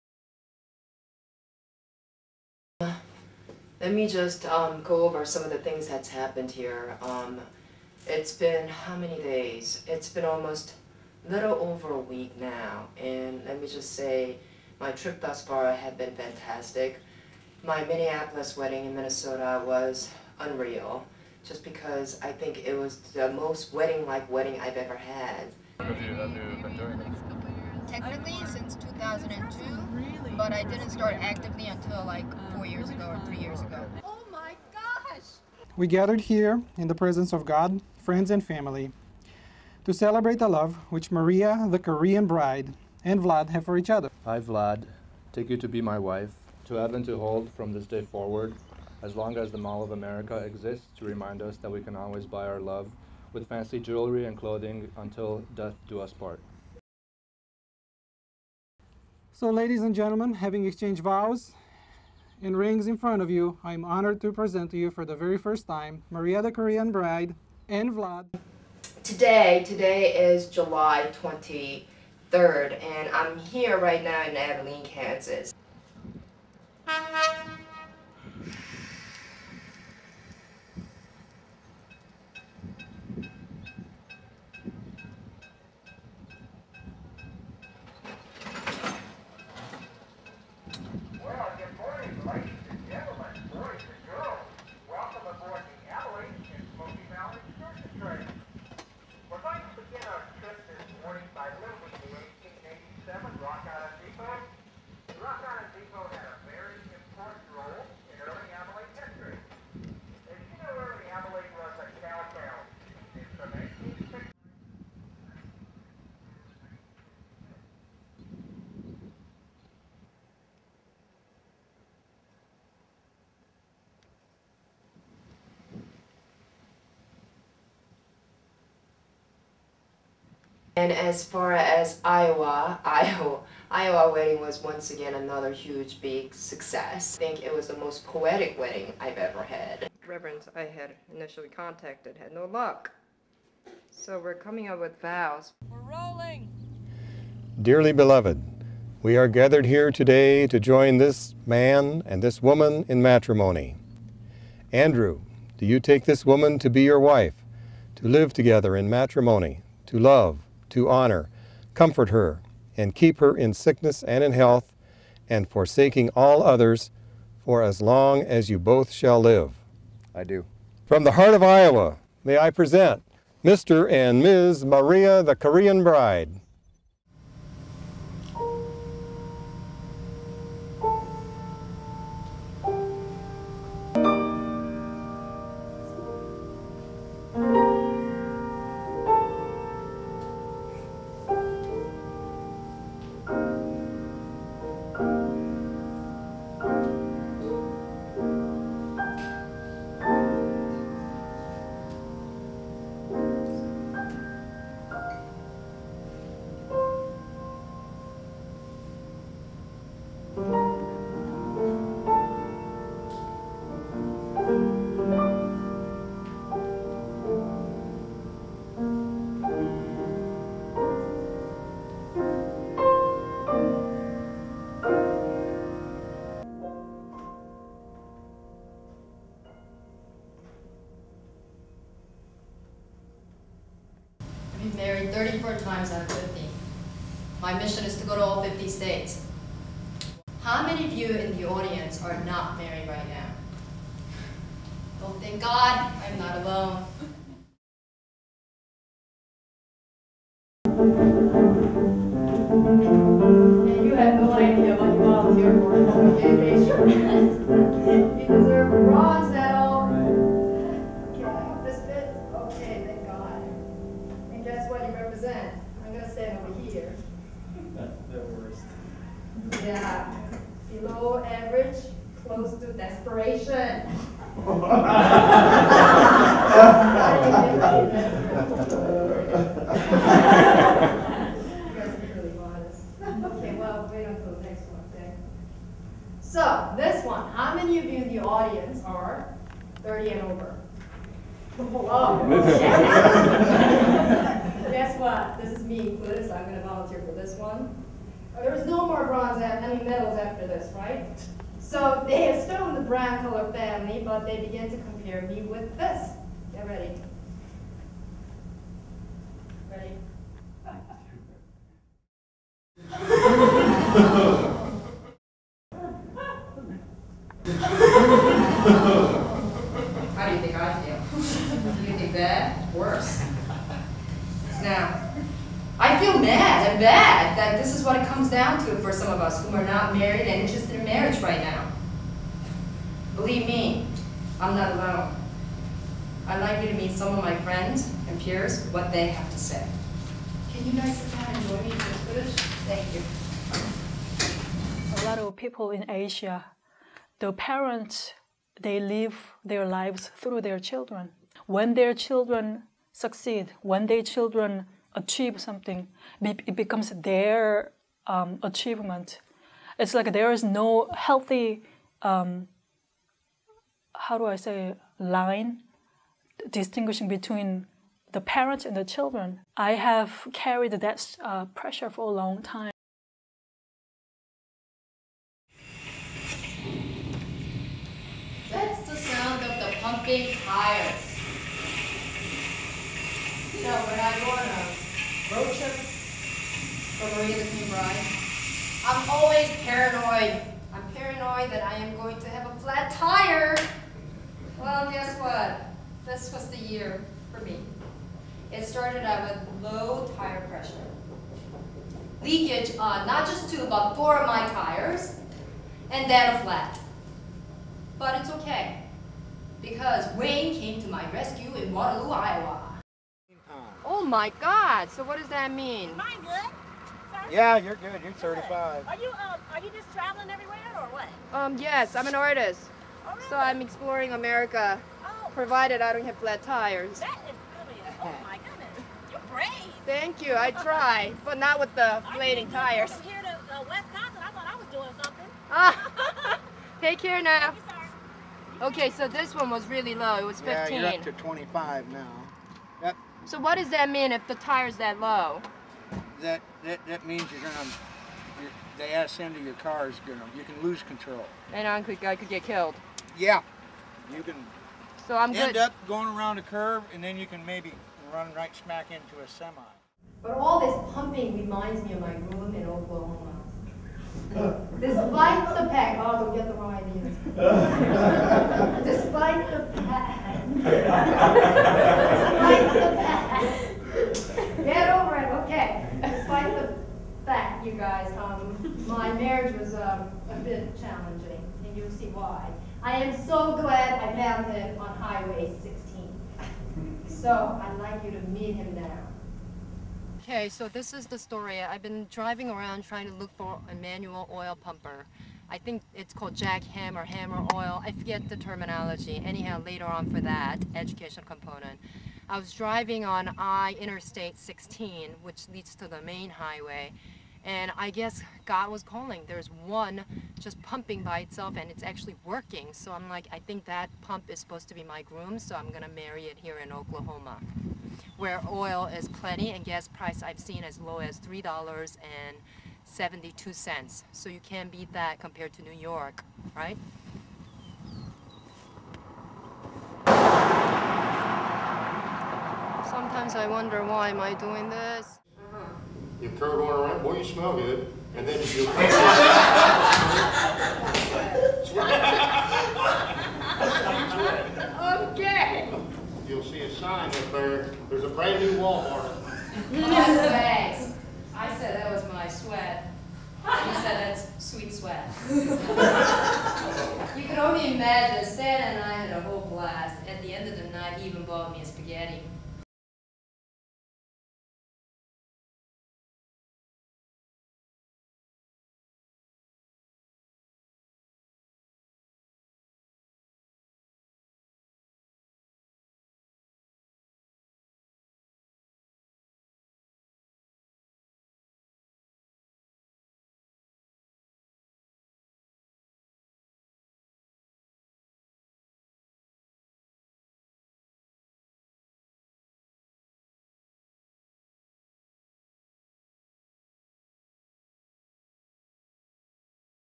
Maria the Korean Bride Live on Stage, Recital Hall, Abrons Arts Center, 2008
Off Off Broadway, 10 min version of Maria the Korean Bride Live Performance on Stage, Recital Hall, NYC. Directed by Sara Shives. Entire Play: 70 min.